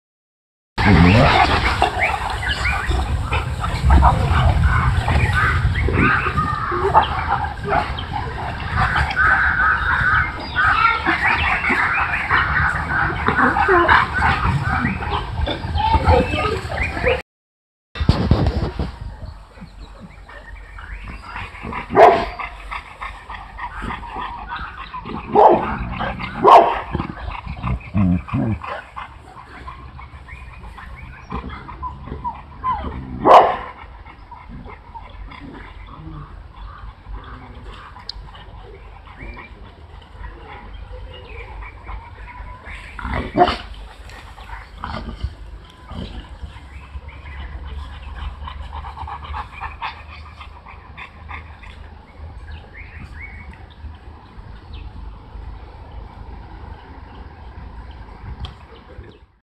Real Boerboels - Playful Moments of Big Boerboel Mummas: Playtime and Resting
Experience the heartwarming bond of big Boerboel mamas as they indulge in playful antics and soothing moments of rest. These magnificent canine mums showcase their nurturing instincts as they engage in joyful playtime, reminding us of the joy and energy that motherhood brings.

Watch as these gentle giants share affectionate moments, from lively play sessions to serene resting periods. Join us in celebrating the remarkable love and companionship shared between these remarkable Boerboel mamas, a testament to the beauty of canine motherhood.